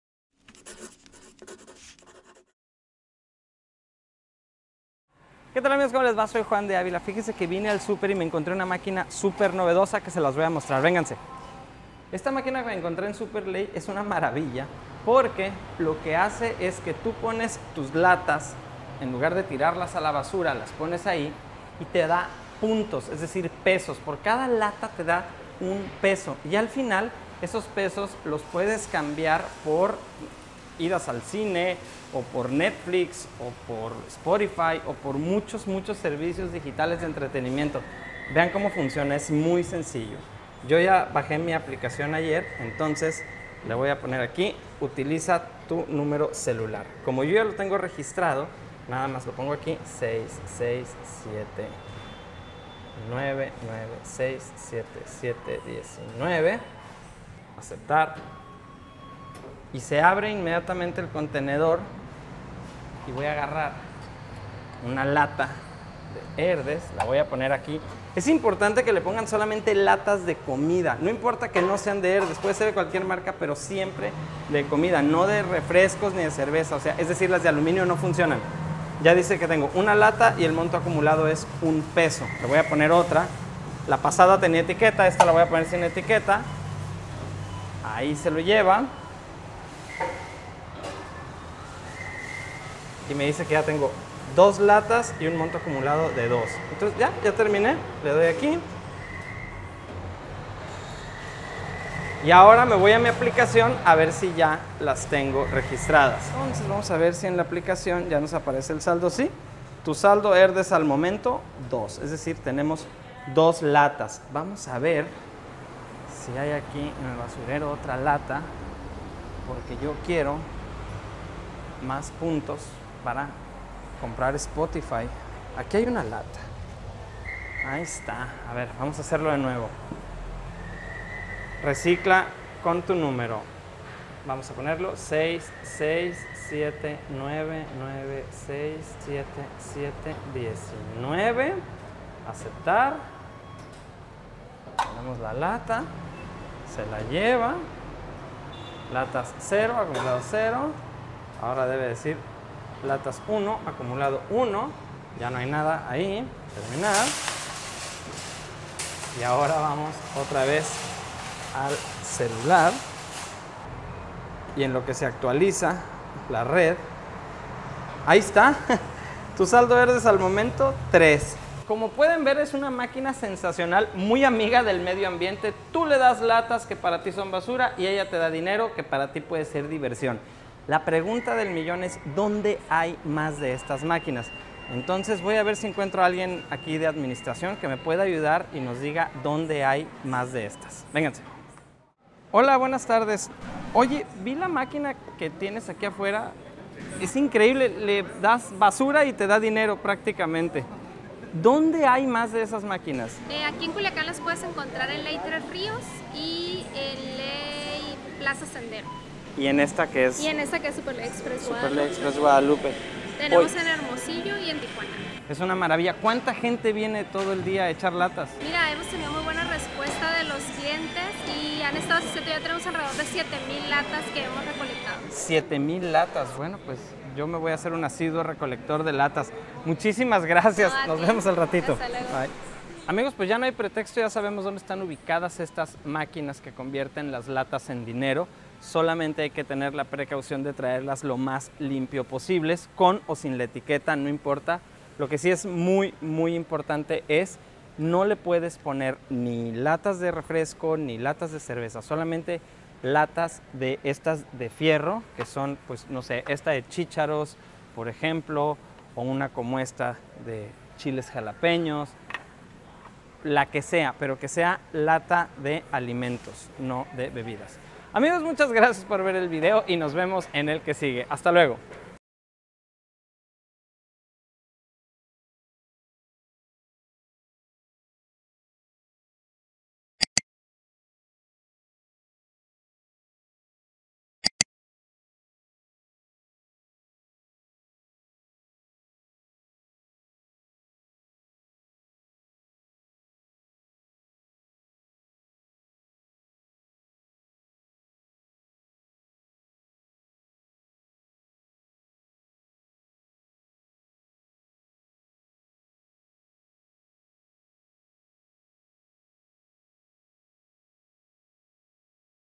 Te dan dinero por tus latas vacías !!!!
Casa Ley y Herdez pusieron unas máquinas recicladoras de latas, el cual otorga dinero o puntos que son canjeables desde una aplicación o la misma maquina para el pago de servicios digitales.